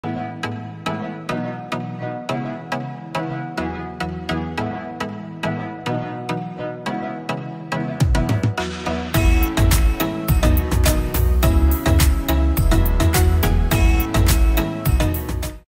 Painting Hacks With Hammer | Abstract Art | Crafty Koala
Were smashing hearts… literally! Create the most unique abstract, heart painting with thid painting technique. 💗🎨🔨